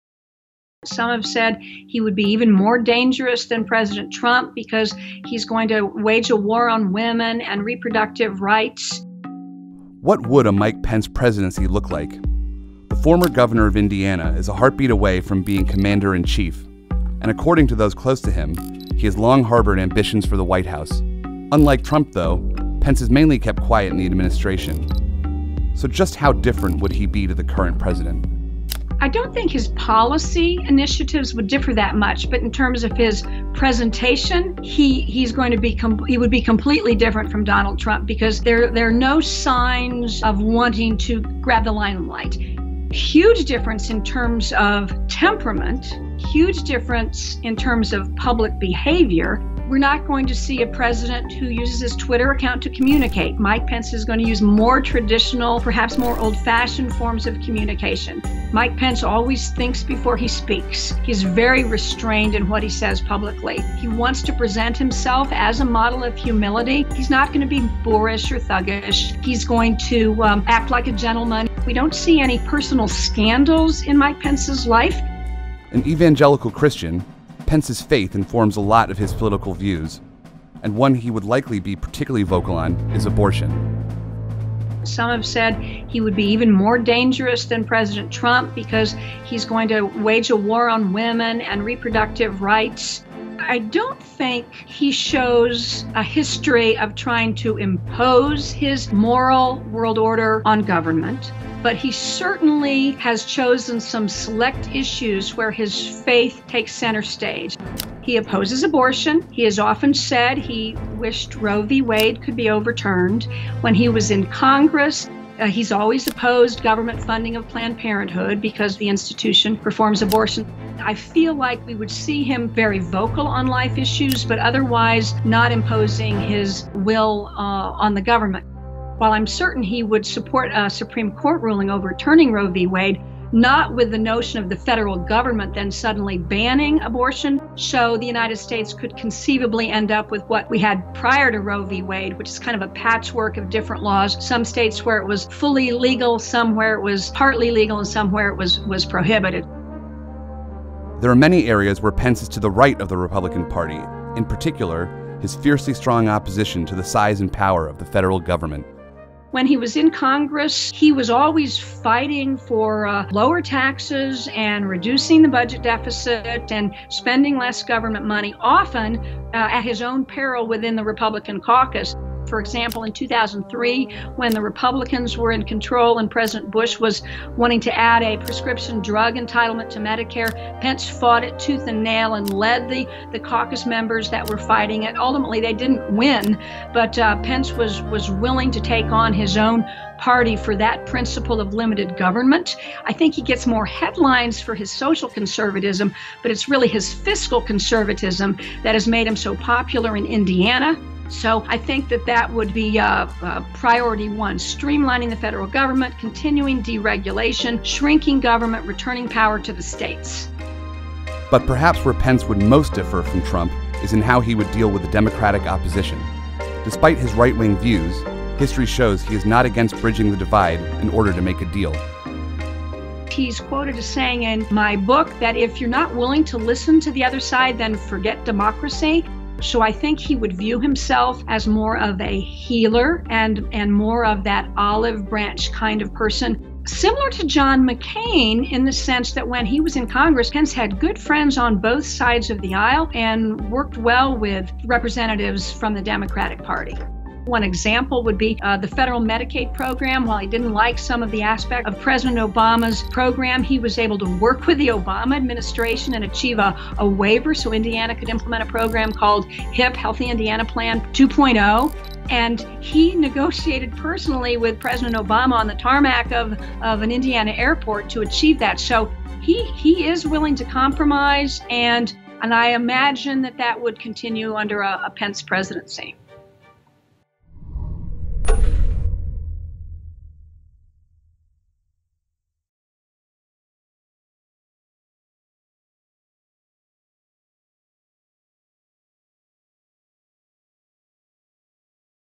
What A Mike Pence Presidency Would Look Like
The former governor of Indiana is a heartbeat away from being commander in chief.
And according to those close to him, he has long harbored ambitions for the White House.
What would a Mike Pence presidency look like?